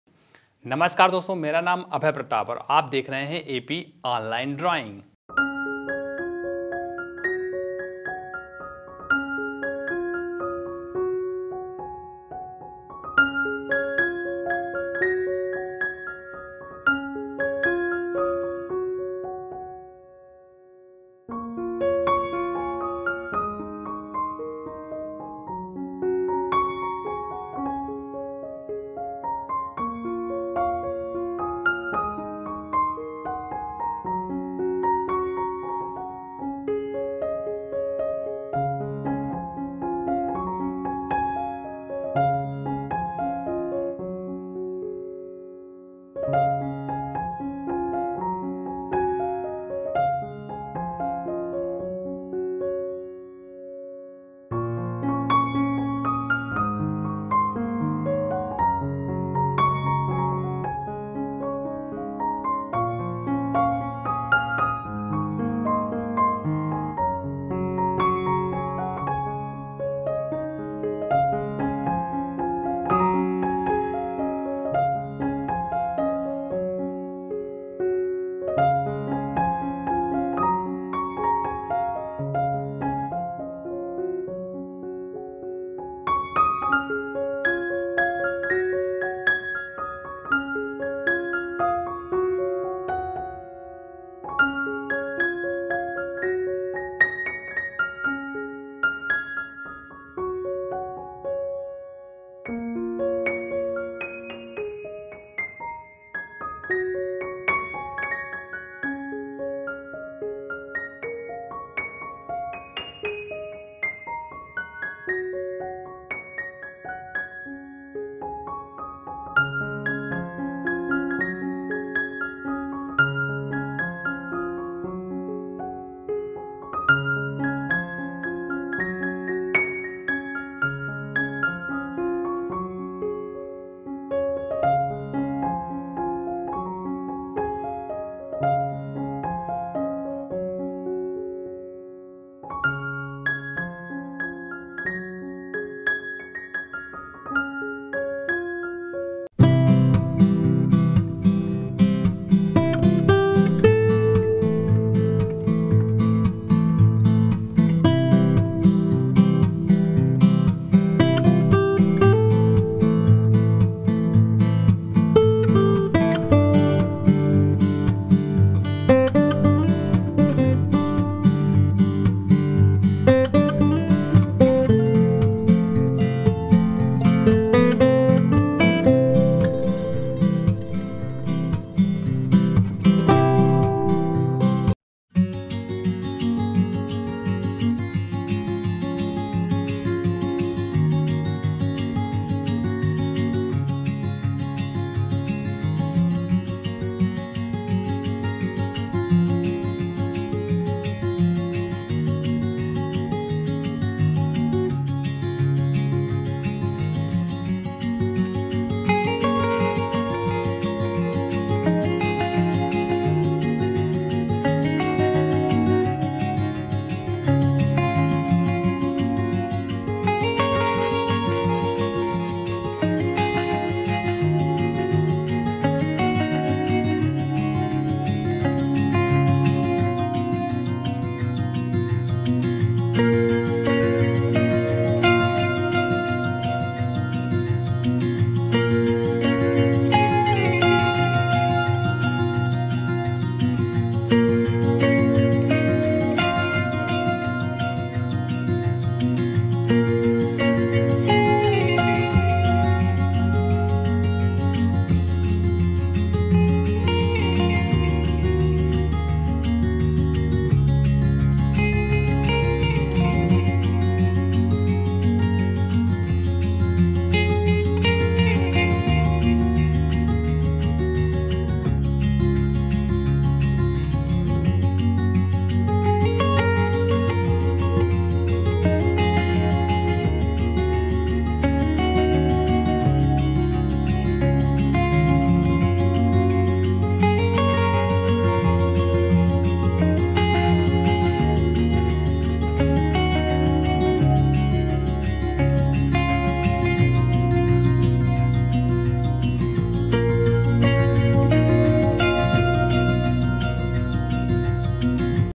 Special Valentine's day Drawing | step by step easy Drawing | love hibiscus Flower Drawing
today I am show how to Draw a valentine's day Special Drawing for beginners